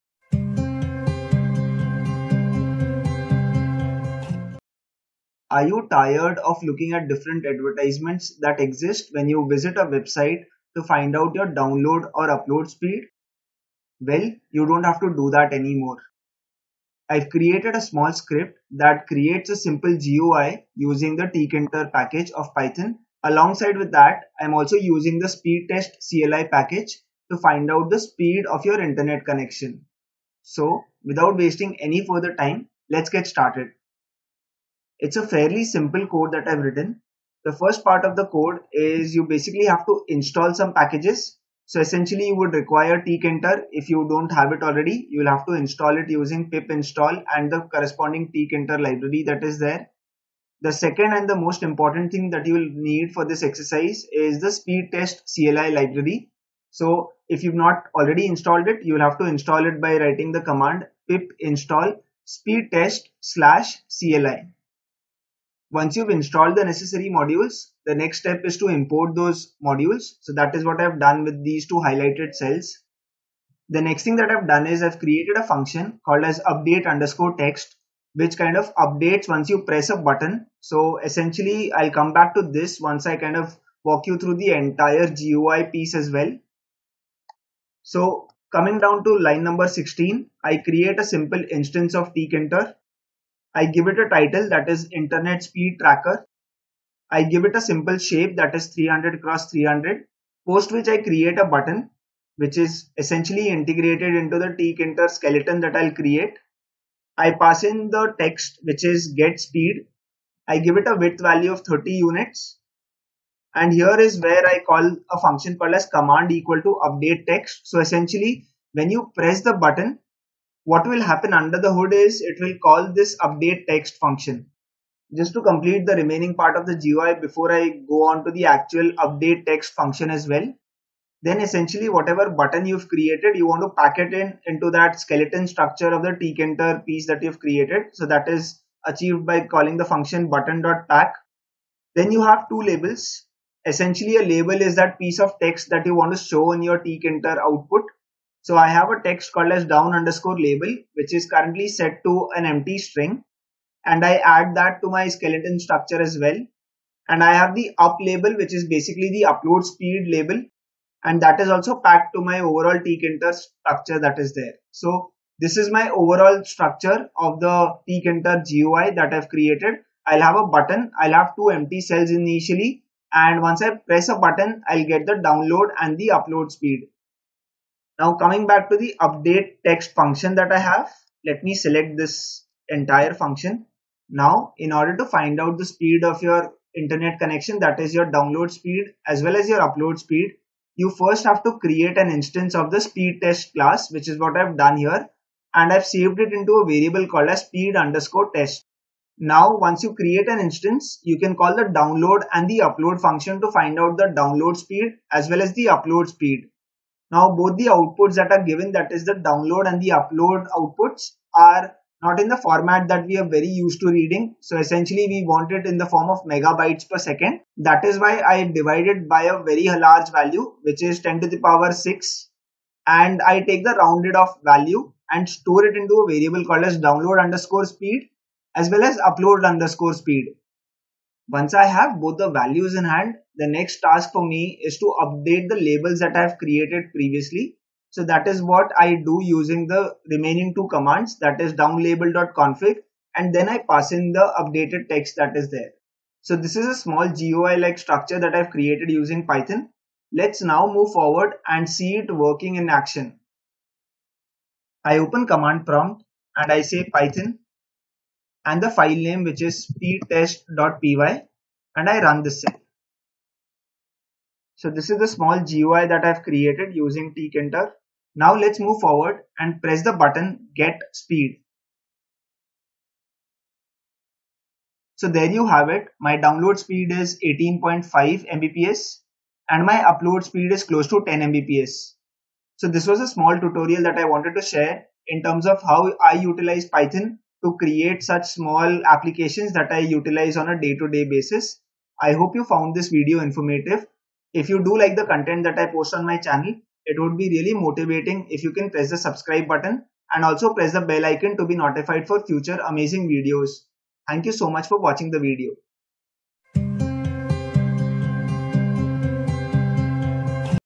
Test Internet Speed using Python (Tkinter & speedtest-cli)
In this video, I'll show you how you can create a simple GUI using Python's speedtest package & Tkinter to examine the internet upload speed & download speed!